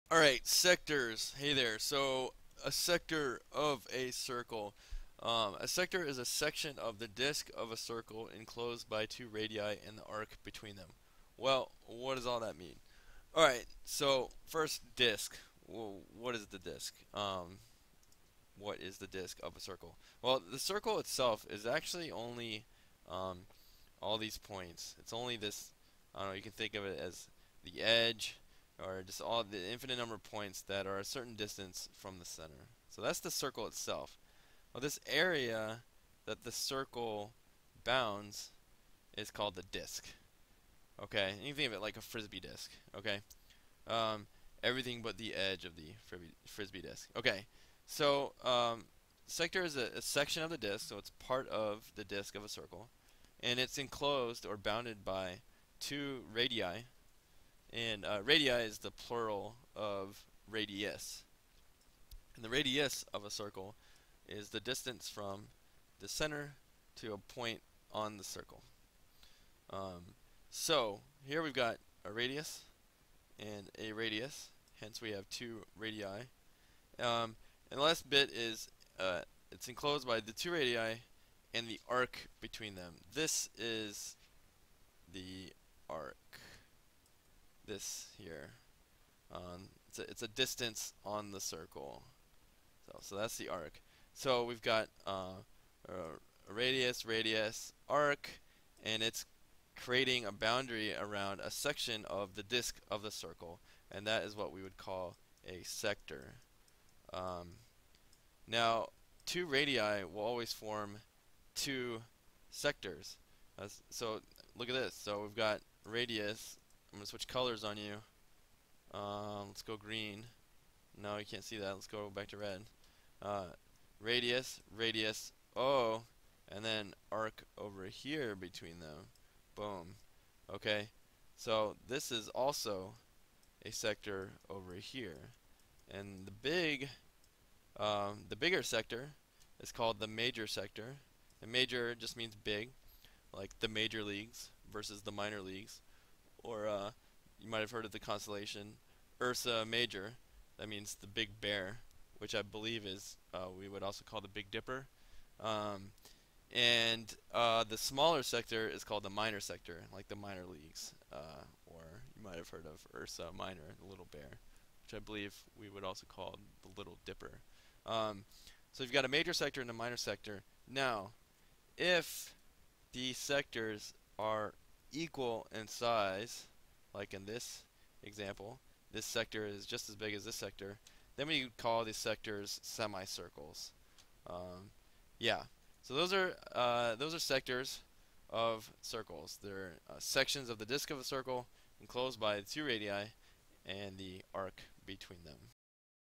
What a Sector of a Circle Is
This is what the concept of a "sector" is, as it relates to circles!